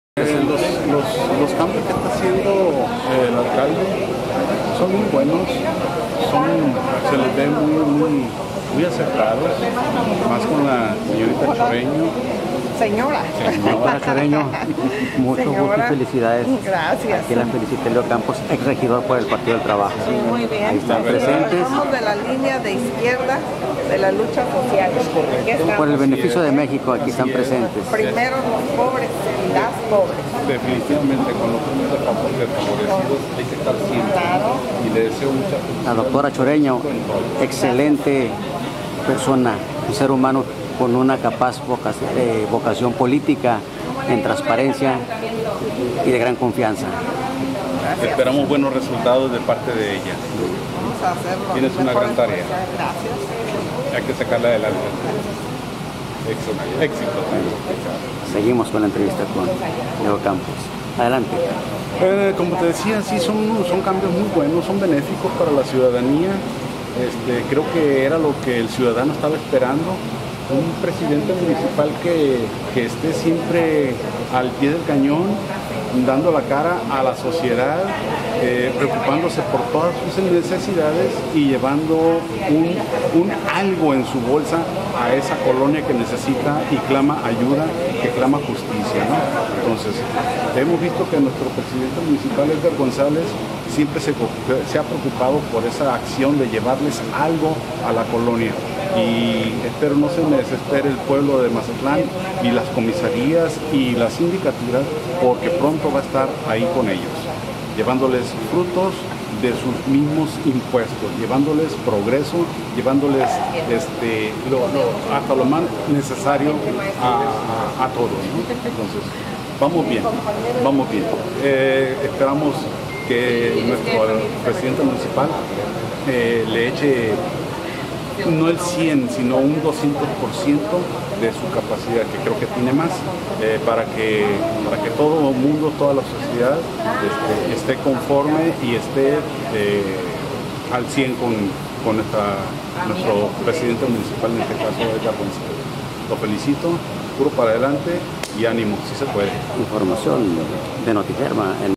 LEO CAMPOS EX-EDIL DIJO, BUEN TRABAJO DEL ALCALDE EDGAR GONZALEZ
MAZATLAN, SINALOA, TAMBIEN PLATICO CON LA NUEVA DIRECTORA DE INMUJER